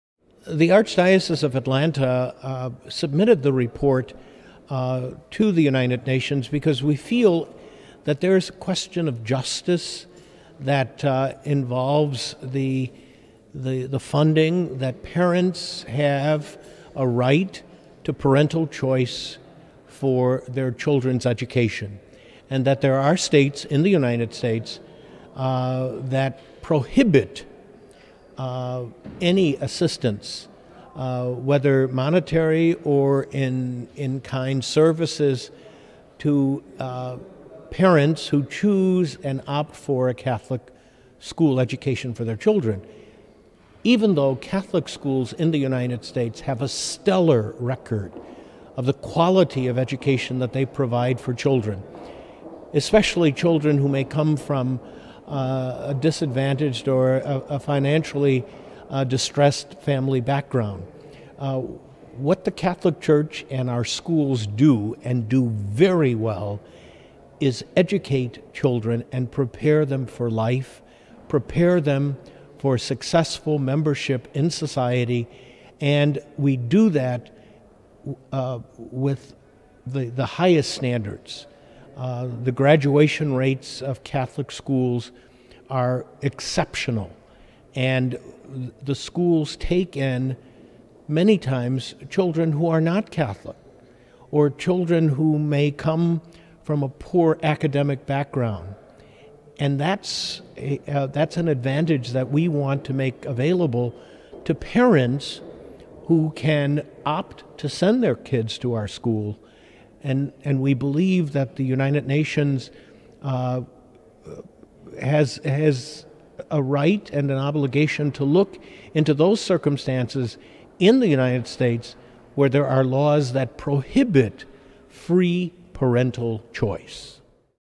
Archbishop Wilton D. Gregory of Atlanta at the U.N. Geneva about Free Parental Choice
During todays (November 24th, 2015) Committee for the Convention on the Elimination of all forms of Racial Discrimination, at the U.N. Geneva, Palais Wilson, H.E. Archbishop Wilton D. Gregory of Atlanta submitted a report to the committee for free parental  choice of schools.

We asked the Archbishop about the reason, purpose and content of his report.